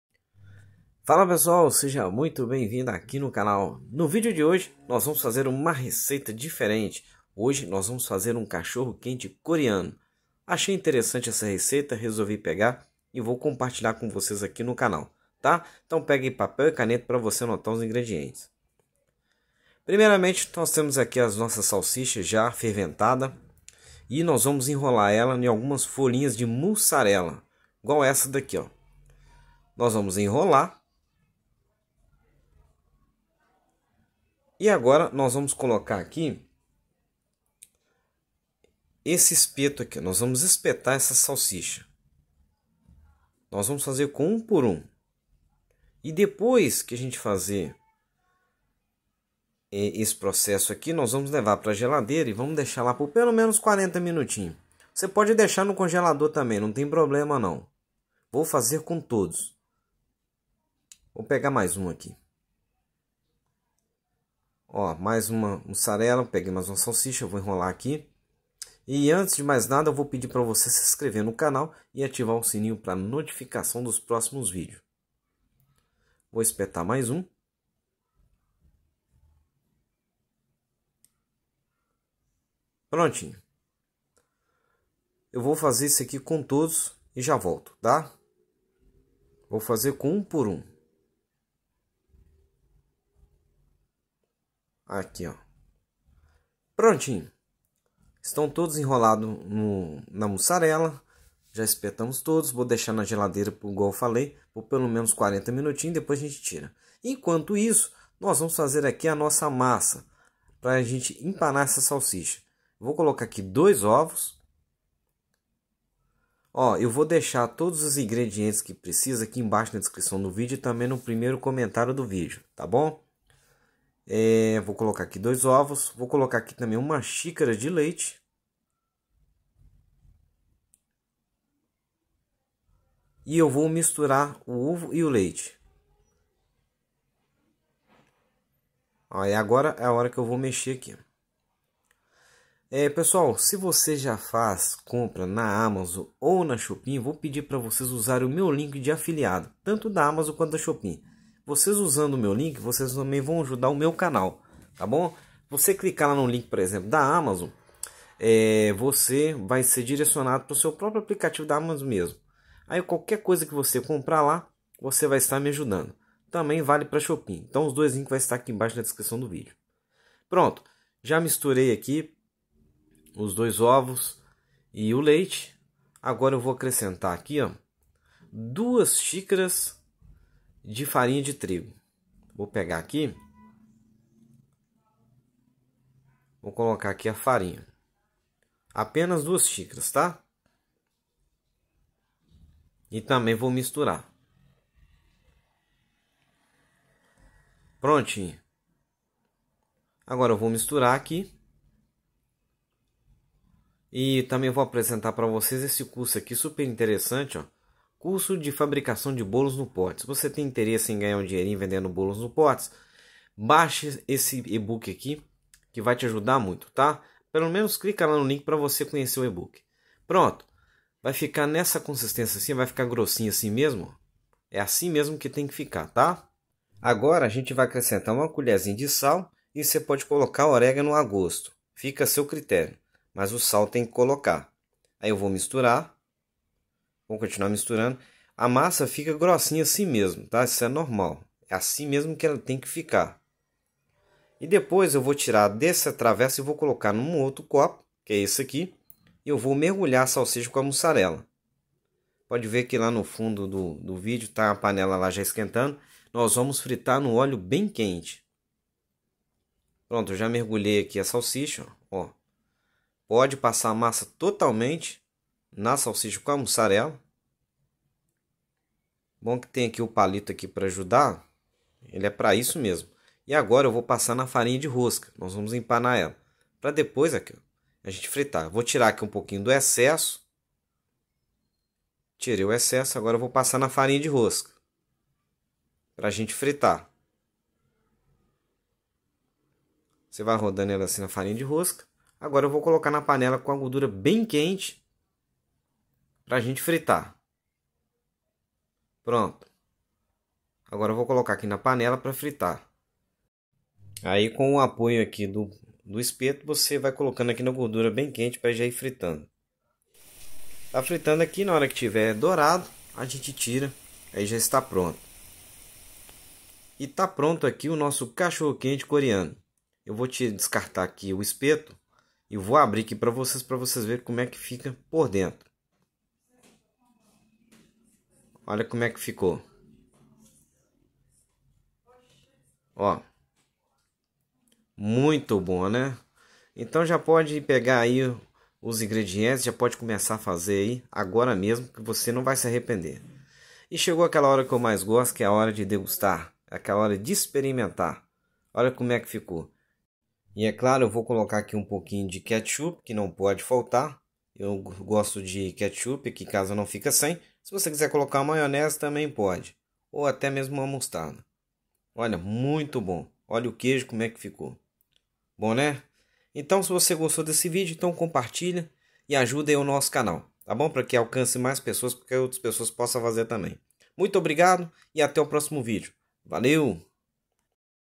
OLHA HOT DOG COREANO SUPER FÁCIL.
Como  fazer essa deliciosa receita de hot dog coreano.
Ingredientes:
* 400 g de salsinha
* 300 g Mussarela
* Espertinho
* 2 ovos
* 1 xícara de leite
* 2 xícaras de farinha de trigo
* 1 Colher raza de sal

📢 BAIXE O CURSO COMPLETO DE FABRICAÇÃO DE BOLOS NO POTE 🍰

00:00  Receita de hot dog  coreano
00:18 Preparo das salsichas
01:48 Preparo da massa
04:06  Empanando as salsichas para fritar
05:18 Pronto para servir